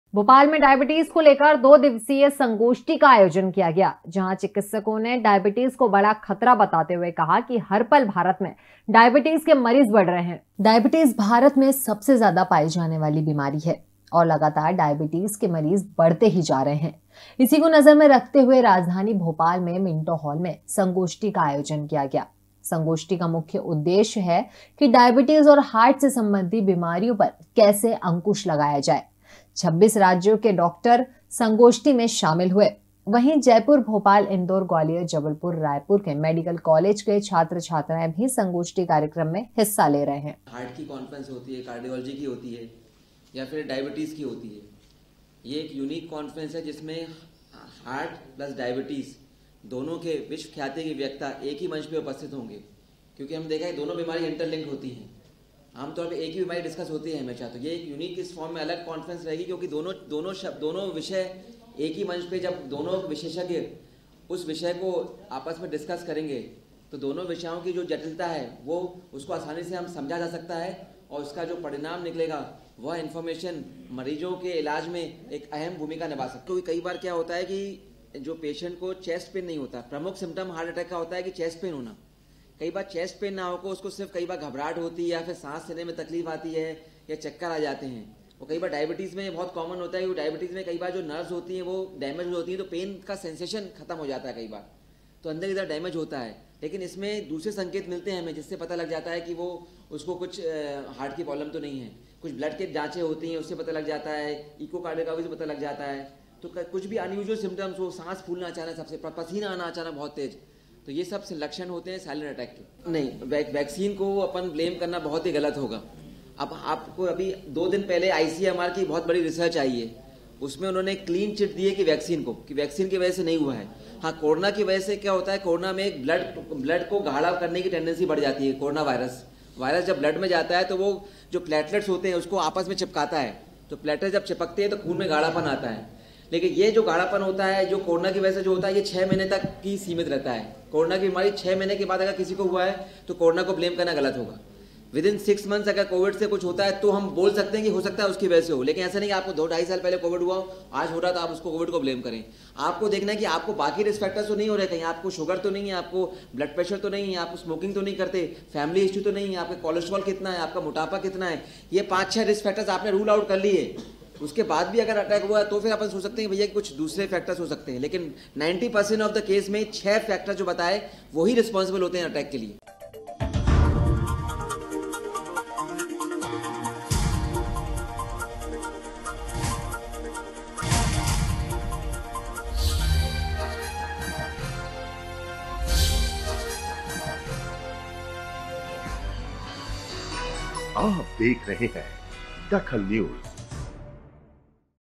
DAKHAL NEWS लगातार बढ़ रहे डायबिटीज के मरीज
लगातार बढ़ रहे डायबिटीज के मरीज
राजधानी भोपाल में  संगोष्ठी का आयोजन